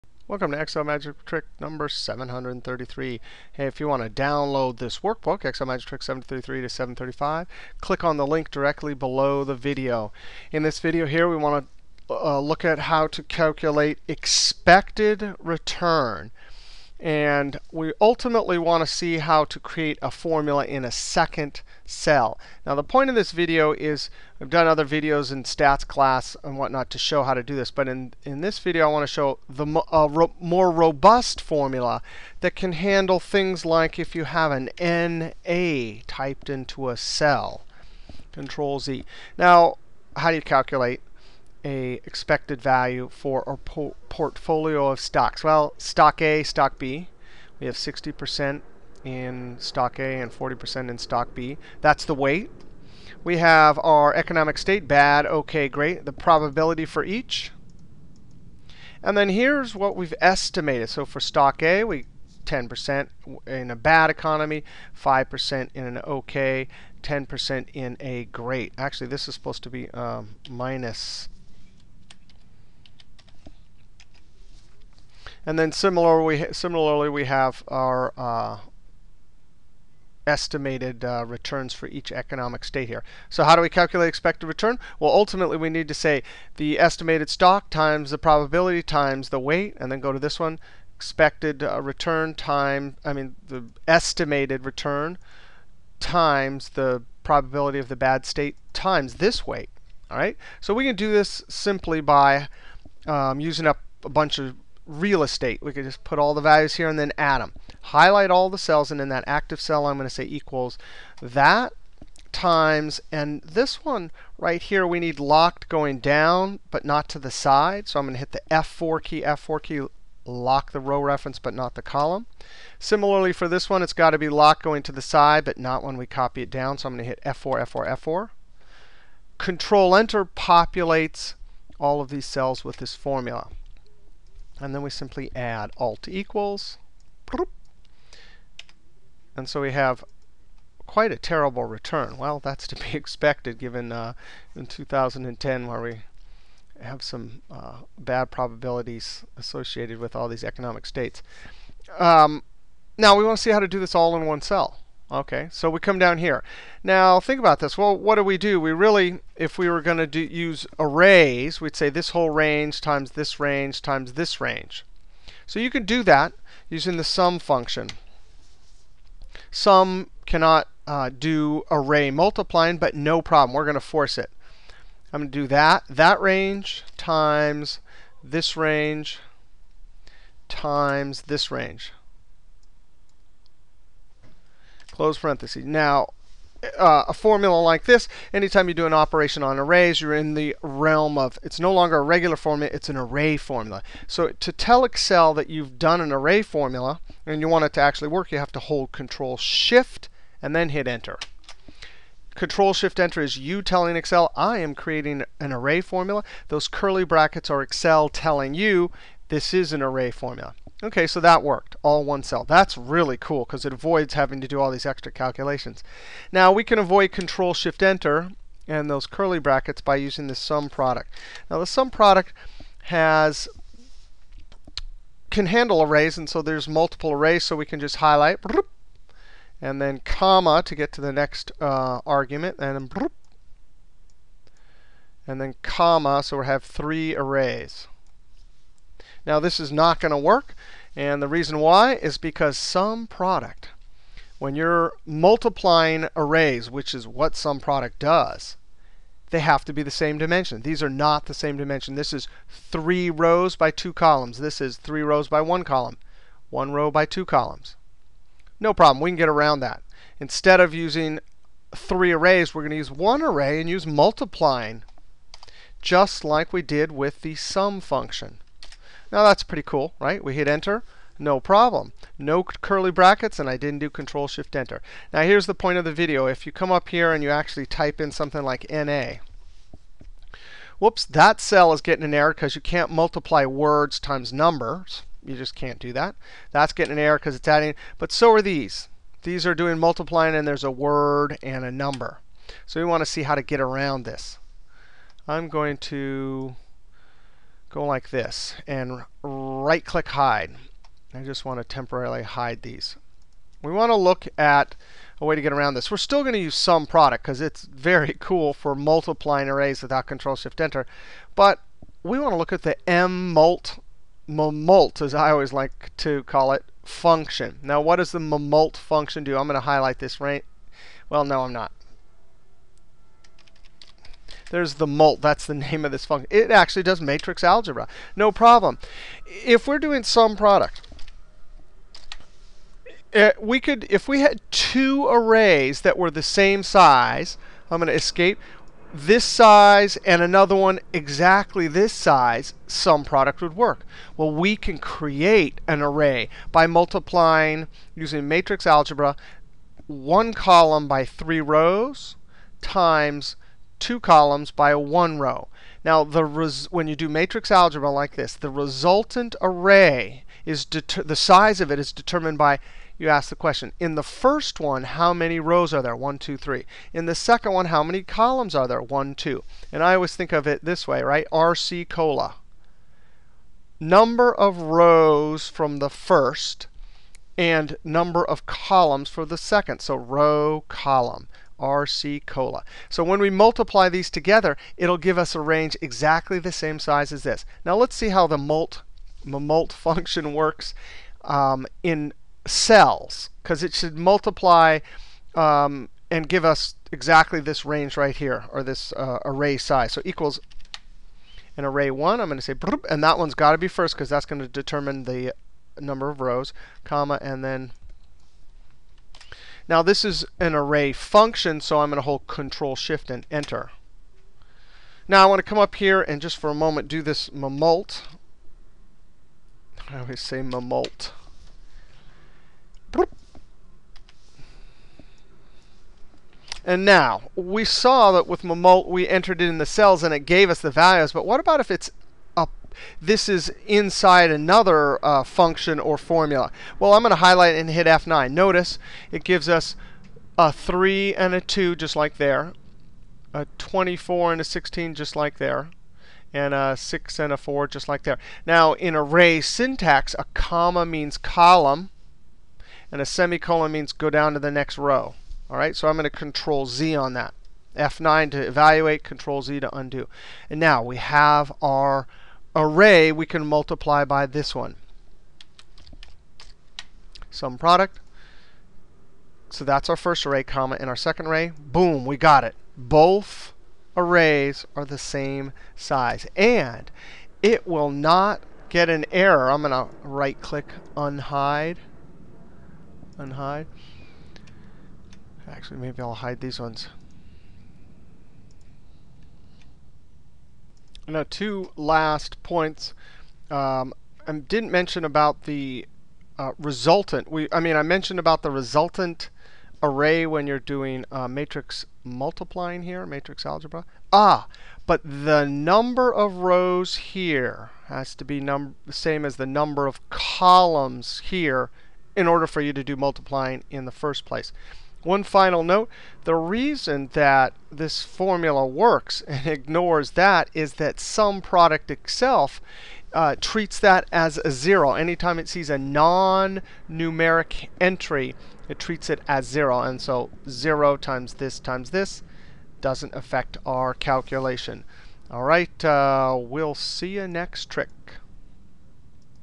Excel Magic Trick 733: Expected Returns For Portfolio SUMPRODUCT & MMULT functions
Learn how to create a robust formula that calculates Expected Returns For Portfolio using thw SUMPRODUCT & MMULT functions.